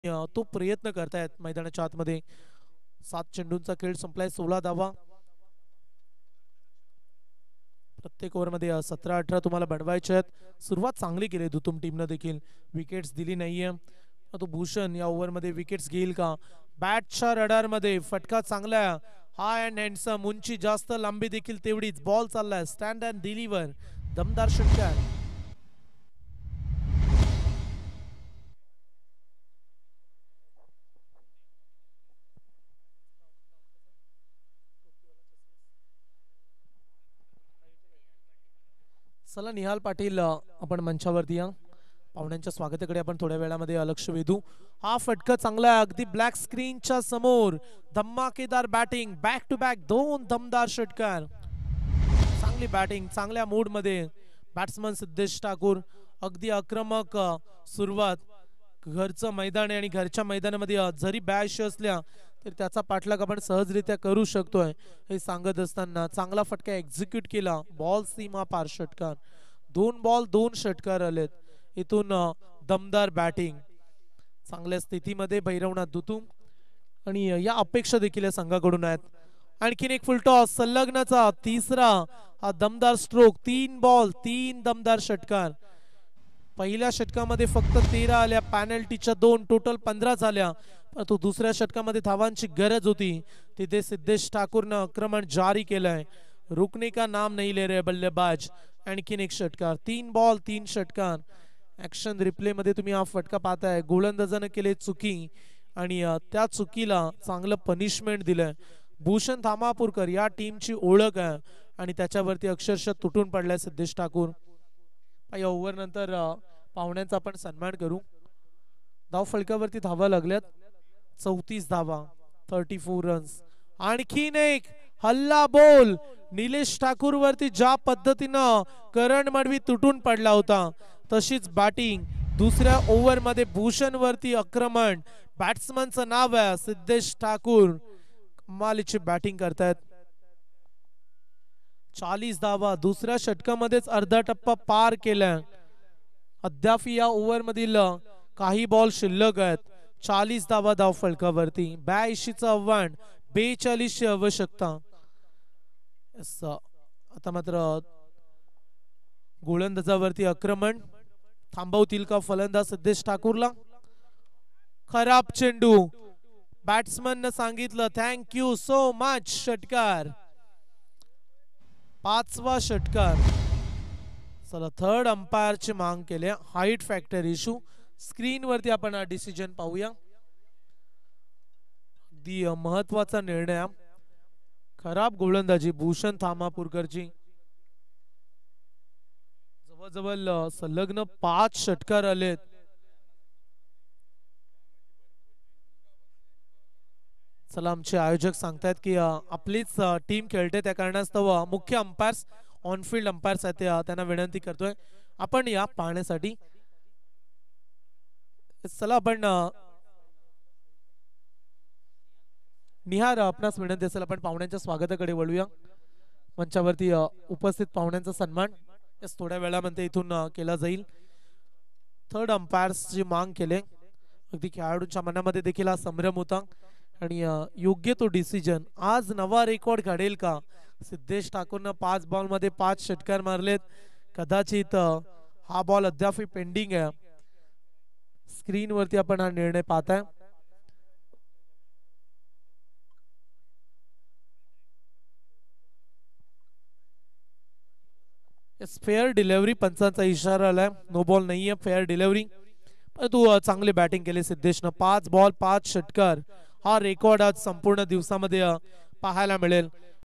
Siddhesh Thakur (Dhutum) | 5 Balls 5 Six | Bhairavnath Chashak 2024 | Dhutum,Uran
Bhairavnath Chashak 2024 | Dhutum,Uran